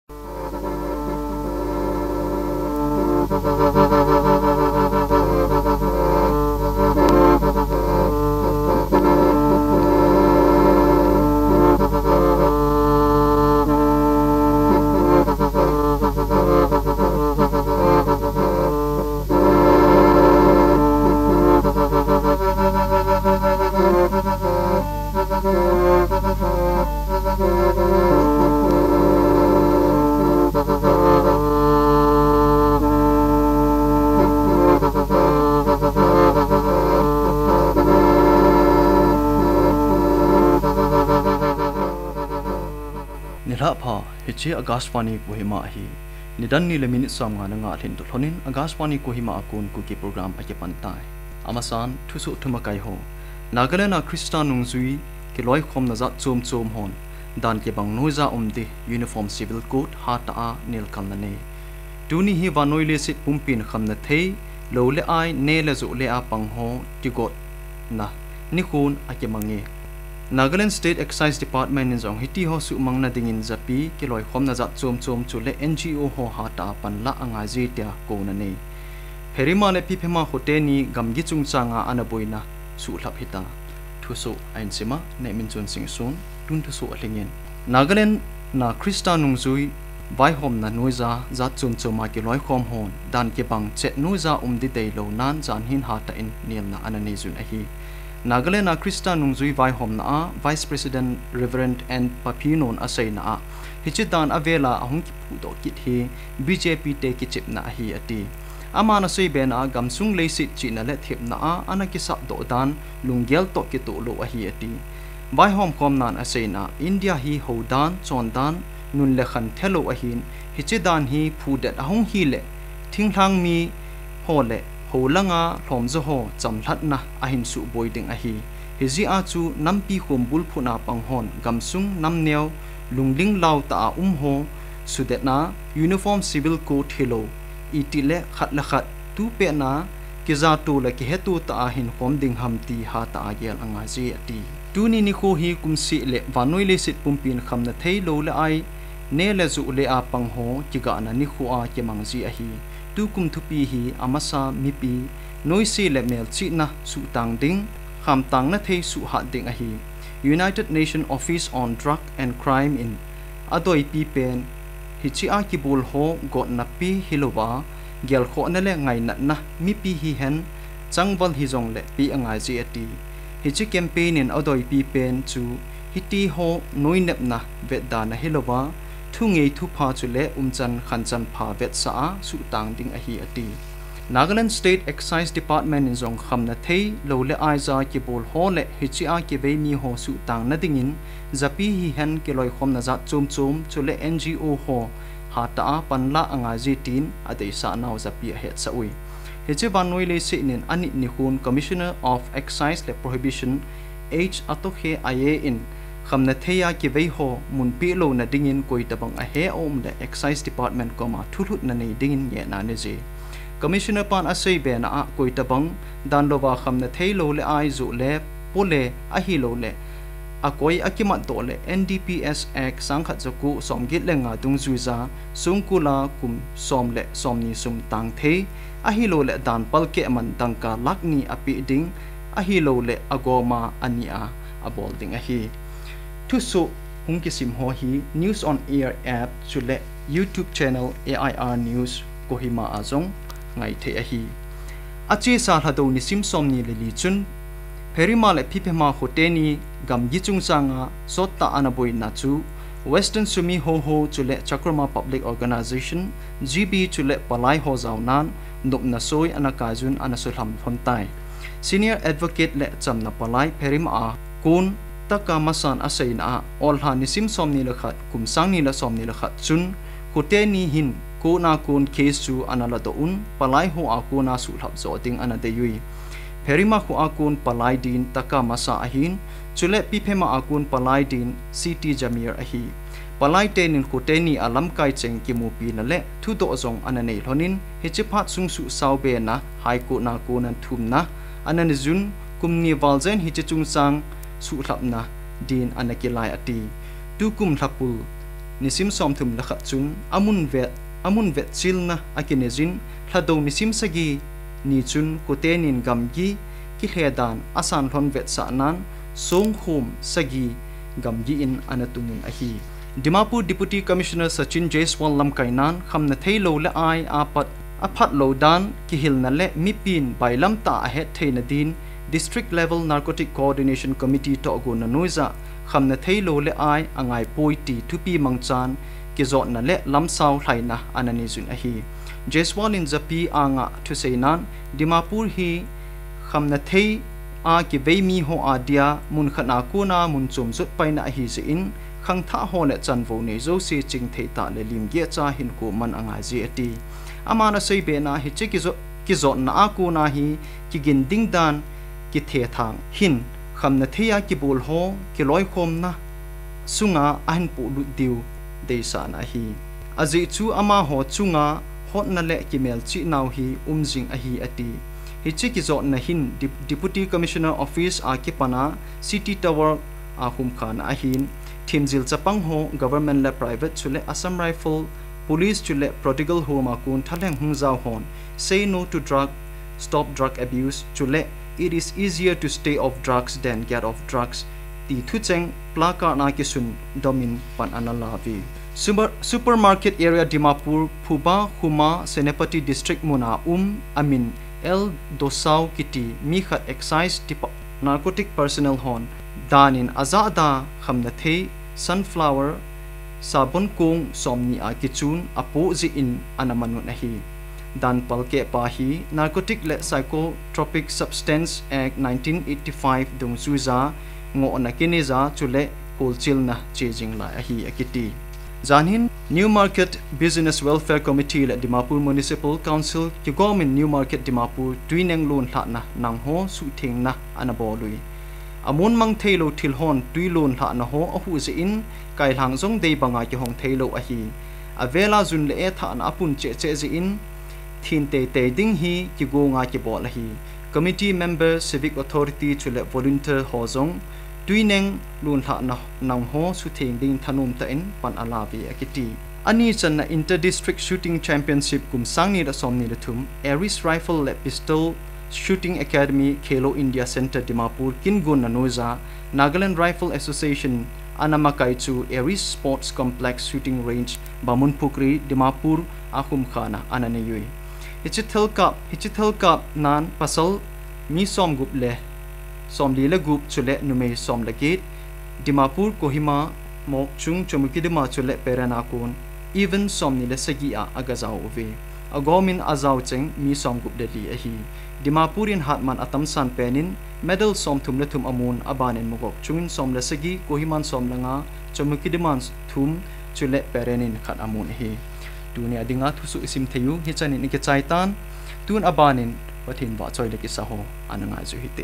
Akashvani News Kohima Kuki Bulletin on June 26, 2023
::THE HEADLINES::
1. Nagaland Joint Christian Forum comprising of all Christian denomination in Nagaland, expresses strong protest against Uniform Civil Code

2. International Day against Drug Abuse and Illicit Trafficking being celebrated today; Nagaland State Excise department calls for concerted effort of citizens, organizations and NGOs to eradicate menace of drugs from society.

3. And long standing boundary dispute between Pherima and Piphema village finally resolved